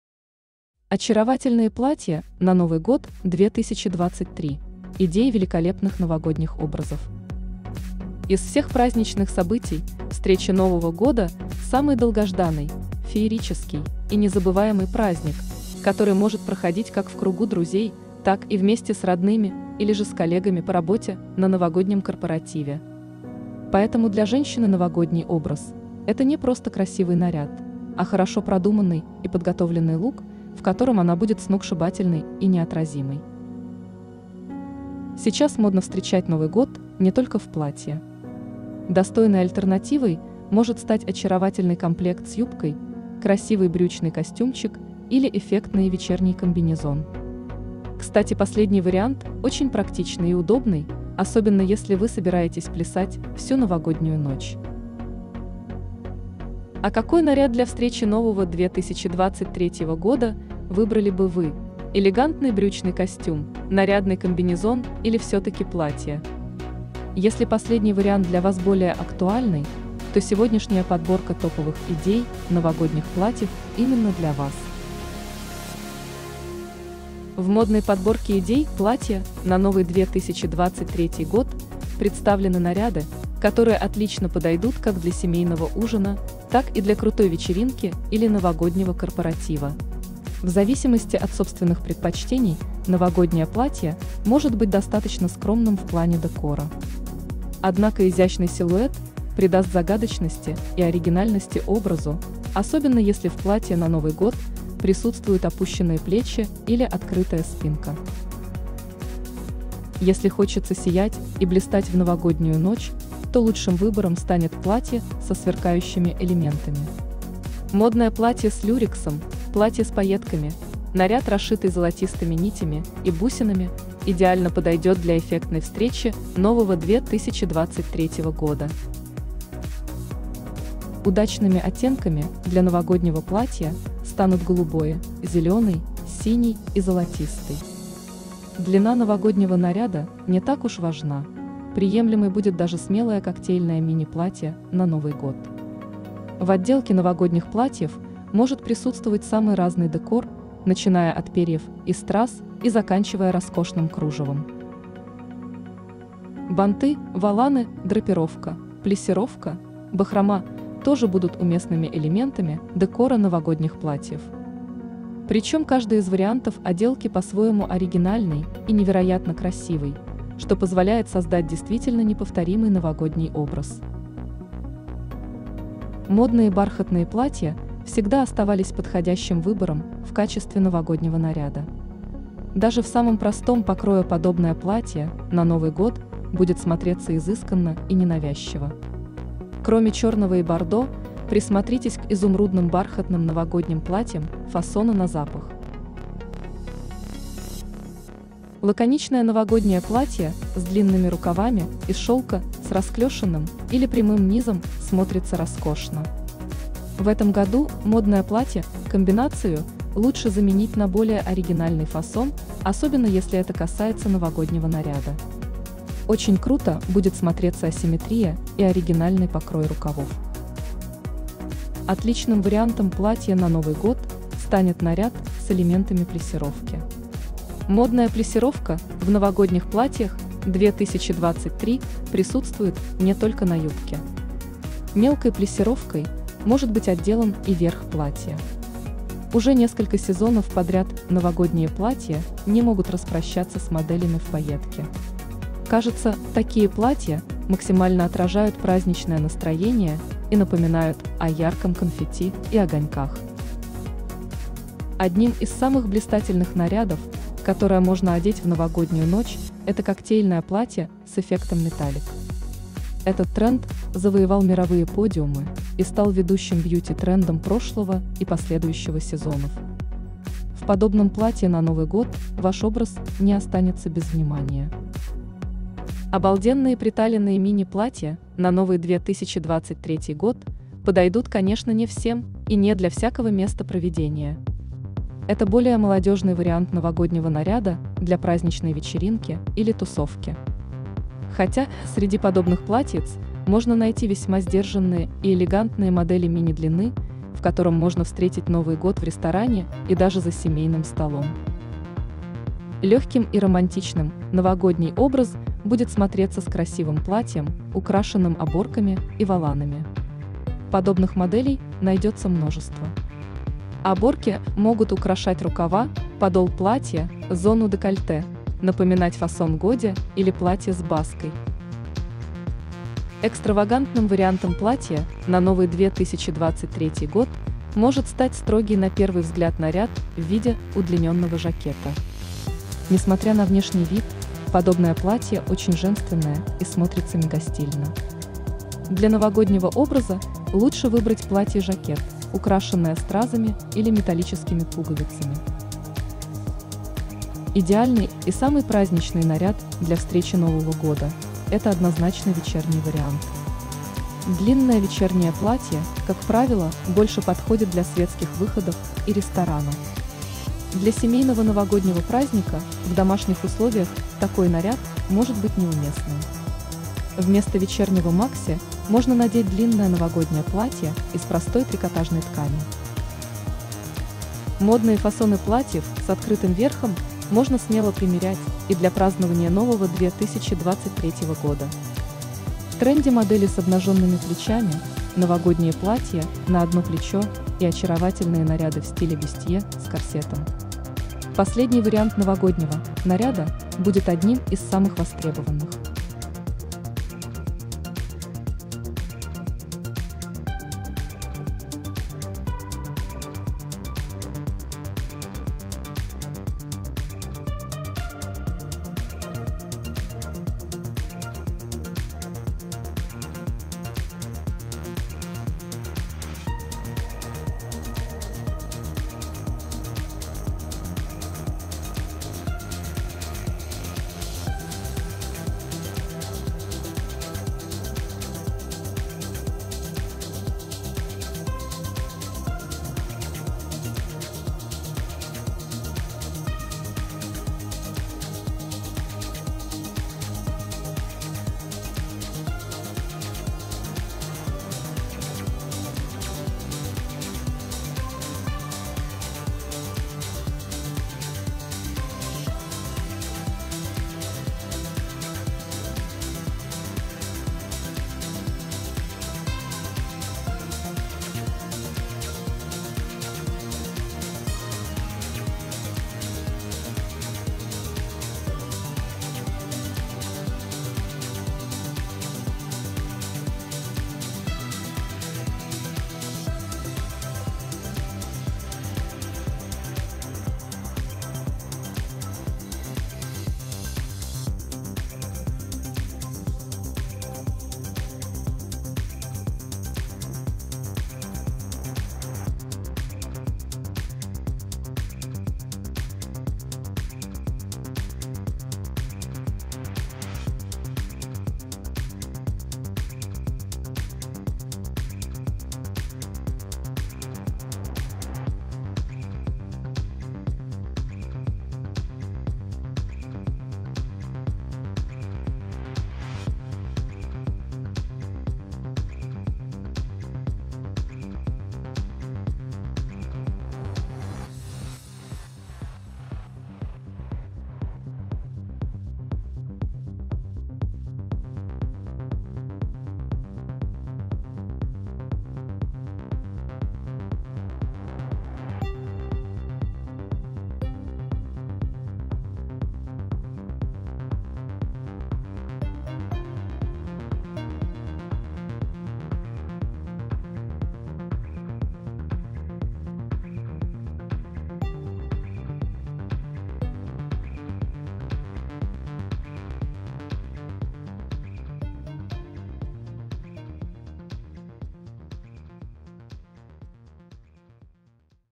Очаровательные Платья на Новый Год 2023
ОЧАРОВАТЕЛЬНЫЕ ПЛАТЬЯ НА НОВЫЙ ГОД 2023: ИДЕИ ВЕЛИКОЛЕПНЫХ НОВОГОДНИХ ОБРАЗОВ
Из всех праздничных событий встреча Нового года самый долгожданный феерический, и незабываемый праздник, который может проходить как в кругу друзей, так и вместе с родными или же с коллегами по роботе на новогоднем корпоративе.
Поэтому для женщины новогодний образ — это не просто красивый наряд, а хорошо продуманный и подготовленный лук, в котором она будет сногсшибательной и неотразимой.
Сейчас модно встречать Новый год не только в платье. Достойной альтернативой может стать очаровательный комплект с юбкой, красивый брючный костюмчик или эффектный вечерний комбинезон. Кстати последний вариант очень практичный и удобный, особенно если Вы собираетесь плясать всю новогоднюю ночь.
А какой наряд для встречи Нового 2023 года выбрали бы Вы: элегантный брючный костюм, нарядный комбинезон или все-таки платье? Если последний вариант для Вас более актуальный, то сегодняшняя подборка топовых идей новогодних платьев именно для Вас.
В модной подборке идей платья на Новый 2023 год представлены наряды, которые отлично подойдут как для семейного ужина, так и для крутой вечеринки или новогоднего корпоратива.
В зависимости от собственных предпочтений новогоднее платье может быть достаточно скромным в плане декора, однако изящный силуэт придаст загадочности и оригинальности образу, особенно если в платье на Новый год присутствуют опущенные плечи или открытая спинка.
Если хочется сиять и блистать в новогоднюю ночь, то лучшим выбором станет платье со сверкающими элементами. Модное платье с люрексом, платье с пайетками, наряд расшитый золотистыми нитями и бусинами идеально подойдет для эффектной встречи Нового 2023 года.
Удачными оттенками для новогоднего платья станут голубой, зеленый, синий и золотистый.
Длина новогоднего наряда не так уж важна, приемлемой будет даже смелое коктейльное мини-платье на Новый год. В отделке новогодних платьев может присутствовать самый разный декор, начиная от перьев и страз и заканчивая роскошным кружевом.
Банты, воланы, драпировка, плиссировка, бахрома тоже будут уместными элементами декора новогодних платьев, причем каждый из вариантов отделки по-своему оригинальный и невероятно красивый, что позволяет создать действительно неповторимый новогодний образ.
Модные бархатные платья всегда оставались подходящим выбором в качестве новогоднего наряда. Даже в самом простом покрое подобное платье на Новый год будет смотреться изысканно и ненавязчиво. Кроме черного и бордо, присмотритесь к изумрудным бархатным новогодним платьям фасона на запах.
Лаконичное новогоднее платье с длинными рукавами из шелка с расклешенным или прямым низом смотрятся роскошно. В этом году модное платье-комбинацию лучше заменить на более оригинальный фасон, особенно если это касается новогоднего наряда. Очень круто будет смотреться асимметрия и оригинальный покрой рукавов.
Отличным вариантом платья на Новый год станет наряд с элементами плиссировки. Модная плиссировка в новогодних платьях 2023 присутствует не только на юбке. Мелкой плиссировкой может быть отделан и верх платья.
Уже несколько сезонов подряд новогодние платья не могут распрощаться с моделями в пайетки. Кажется, такие платья максимально отражают праздничное настроение и напоминают о ярком конфетти и огоньках.
Одним из самых блистательных нарядов, которое можно одеть в новогоднюю ночь это коктейльное платье с эффектом металлик. Этот тренд завоевал мировые подиумы и стал ведущим бьюти-трендом прошлого и последующего сезонов. В подобном платье на Новый год Ваш образ не останется без внимания.
Обалденные приталенные мини-платья на Новый 2023 год подойдут конечно не всем и не для всякого места проведения. Это более молодежный вариант новогоднего наряда для праздничной вечеринки или тусовки. Хотя, среди подобных платьиц можно найти весьма сдержанные и элегантные модели мини длины, в котором можно встретить Новый год в ресторане и даже за семейным столом.
Легким и романтичным новогодний образ будет смотреться с красивым платьем, украшенным оборками и воланами. Подобных моделей найдется множество. Оборки могут украшать рукава, подол платья, зону декольте, напоминать фасон годе или платье с баской.
Экстравагантным вариантом платья на Новый 2023 год может стать строгий на первый взгляд наряд в виде удлиненного жакета. Несмотря на внешний вид, подобное платье очень женственное и смотрится мегастильно. Для новогоднего образа лучше выбрать платье-жакет, украшенное стразами или металлическими пуговицами.